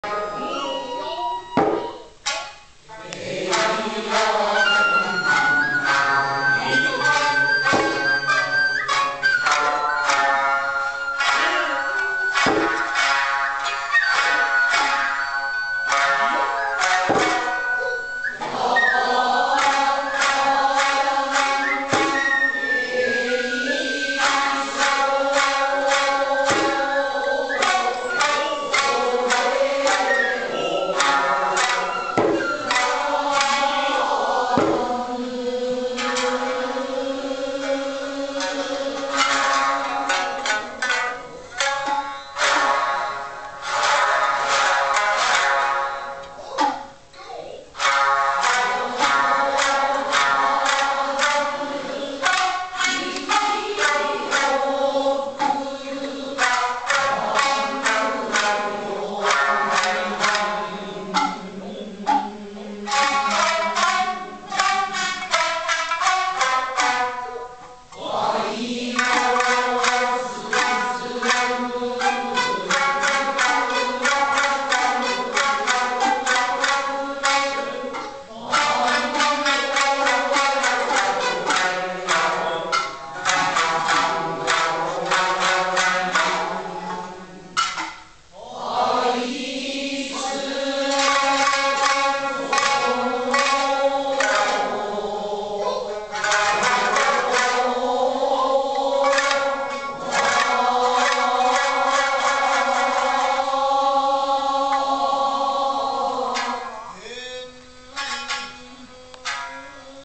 Kine-Ie Concert Rehearsal-Mass Performance
From the rehearsal for the big Kine-Ie concert at the National Theatre in Tokyo in September. My camera's memory card was running low on space, so it's short. This is the first I have of the piece that included all the performers from Kine-Ie that could attend the concert. This includes the Kine-Ie American branch, Kabuki Academy.